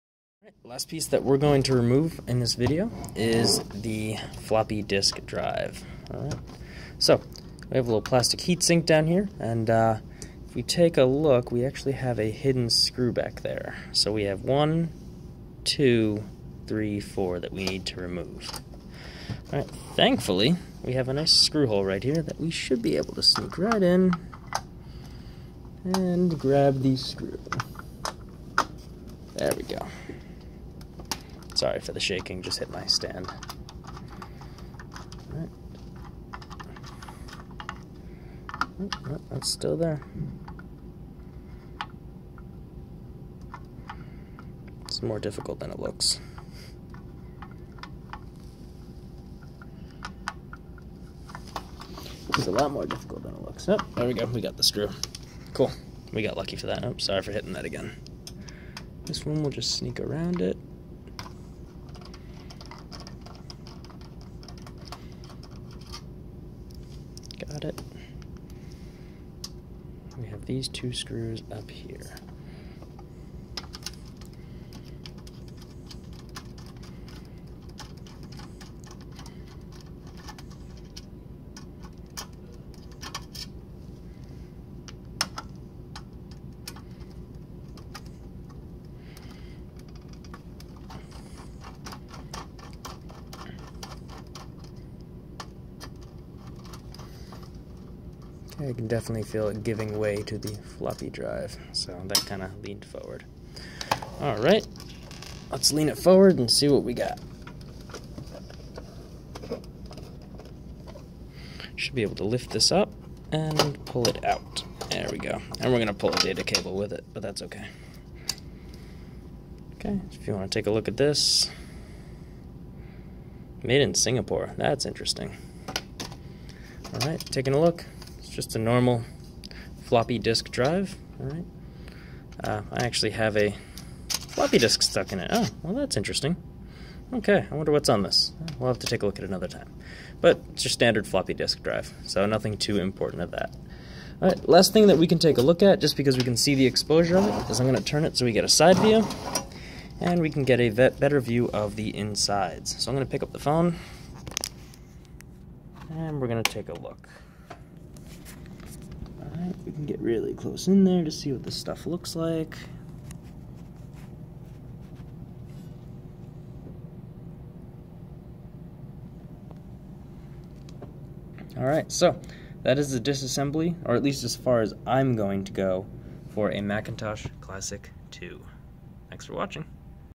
Disassembling a Macintosh Classic II Part 4 - Floppy Drive and Irremovable Components
In this video series, I disassemble a Macintosh Classic II, and all-in-one computer from the 90's.

This is part 4, where I remove the Floppy Disk Drive and we explore the irremovable parts from the device. I do not remove these components for my own personal safety. DO NOT OPERATE ON A POWER SUPPLY OR CRT MONITOR. THE DEVICES CAN HOLD  A CHARGE FOR AN EXTENDED AMOUNT OF TIME. DOING SO CAN RESULT IN SERIOUS INURY OR DEATH.

Leave a Bitcoin Tip: 18qG8g2F9A3nnP5dkyWohDFxN4PhdzGSQ8